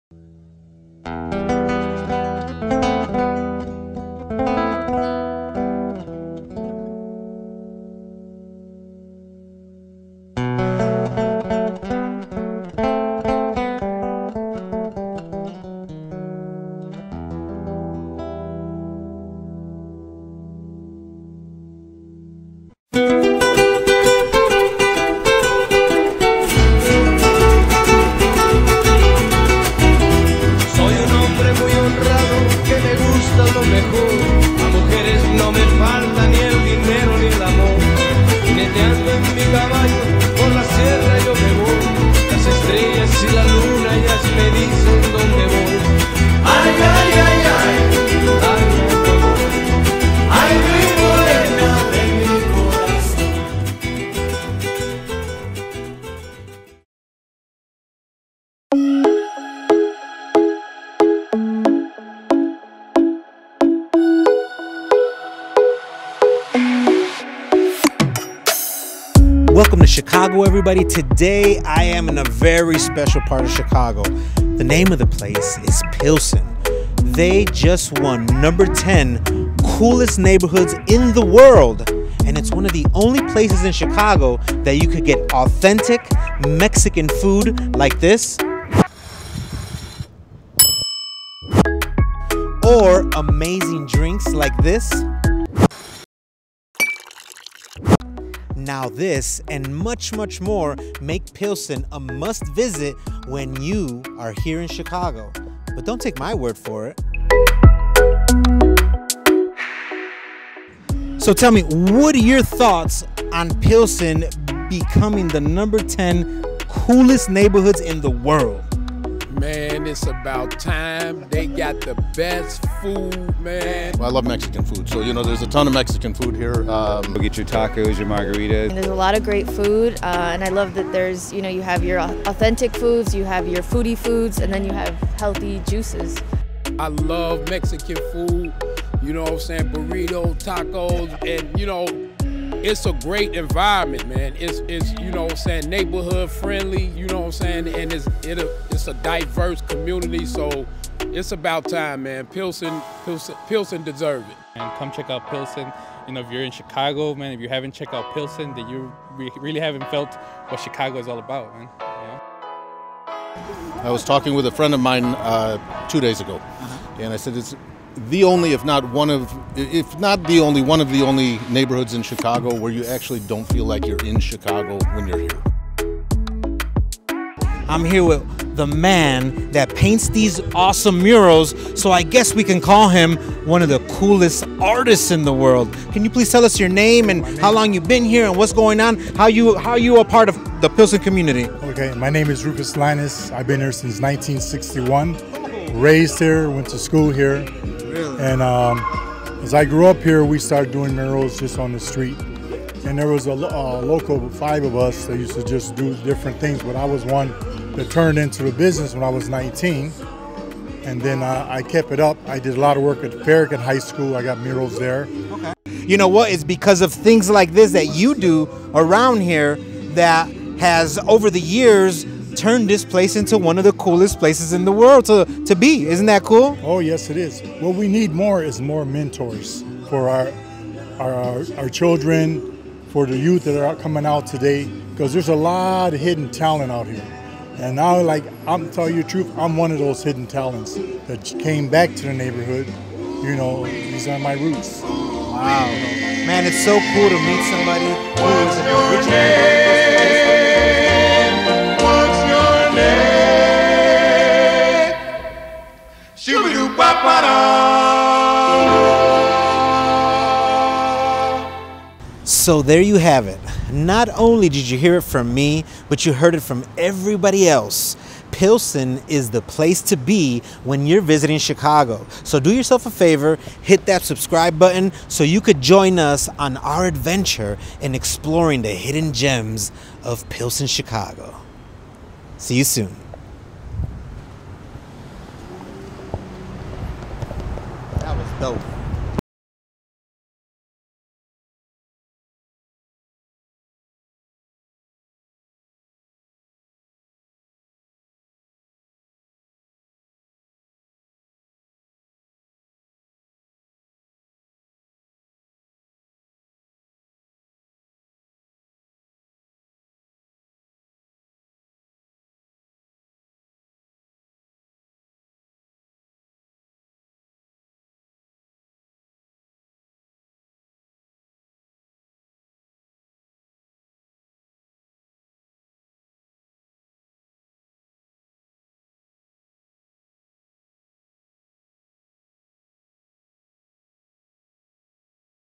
Pilsen Chicago | The Worlds 10 Coolest Neighborhood's Pilsen
Pilsen Chicago | The Worlds 10 Coolest Neighborhood's Pilsen. The Pilsen neighborhood is a historically working class, residential neighborhood, and gateway for immigrants coming to the City of Chicago. The area was initially settled by Czech immigrants to the United States who named the district after Plzeň, a city in what is now the Czech Republic. PILSEN — Chicago’s Pilsen neighborhood is among 12 communities — and only two in the U.S. — featured in Forbes Coolest Neighborhoods Around the World list.
The list, compiled by a Forbes contributor, identifies neighborhoods in major metropolitan cities worth experiencing. Forbes emphasizes that the best way to really know a city is getting “out into the places where local life is really happening.”
The only other U.S. neighborhood recognized alongside Pilsen is Navy Yard in Washington, D.C.
Forbes contributor Ann Abel describes Pilsen as “a nest of cutting-edge culture and art” that features streets lined with galleries and walls decorated with colorful murals dating back to the 1970s.
“It’s been fostered by successive waves of Eastern European and Mexican immigrants, in addition to local artists and students, who have molded this fascinating area over the past century and a half,” the article reads.
The listing points to the National Museum of Mexican Art’s expansive art collection, which highlights the work of Mexican and Mexican-American artists, as well as the growing number of performance venues, art studios, and trendy bars “like the popular Punch House” among the neighborhood’s attractions.
Other cities included in this year’s list are Sants in Barcelona, Amsterdam Noord in Amsterdam, Getsemani in Cartagena, Maboneng in Johannesburg, Plekhanov in Tbilisi, Casco Viejo in Panama City, District 5 in Saigon, Keramikos in Athens, Seongsu-dong in Seoul, and Kalk Bay in Cape Town.